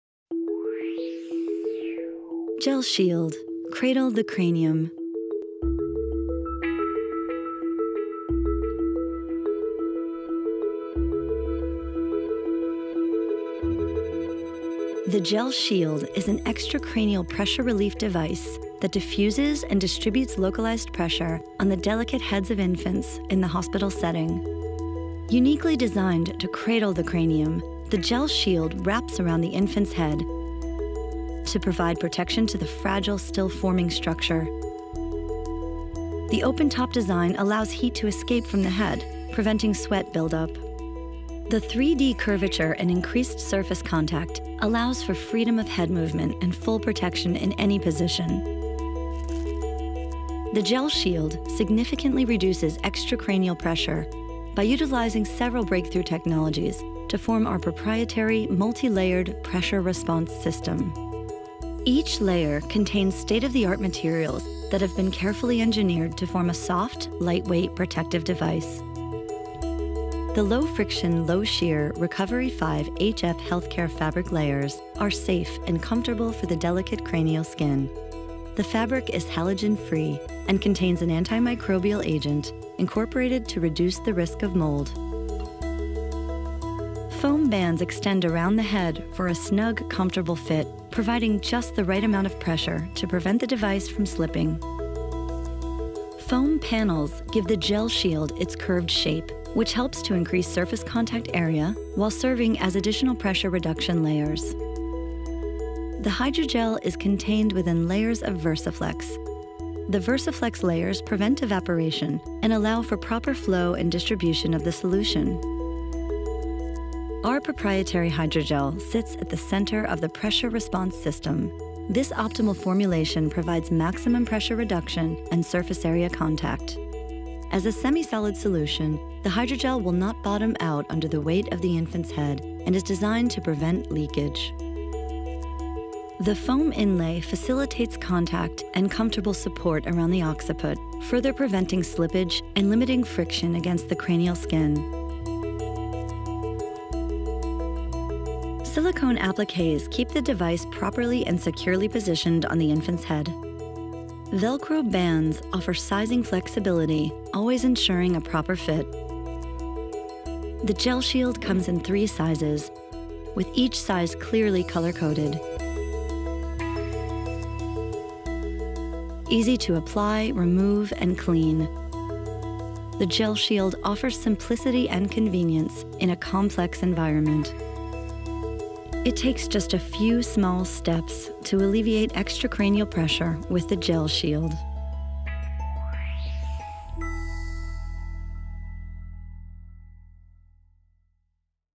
Medical Device Animation: Invictus GELShield
A 3D medical device animation to introduce healthcare professionals to the features of the GELShield extracranial pressure relief system. We developed a soft visual style consistent with GELShield’s purpose and created product renderings for use on the GELShield website.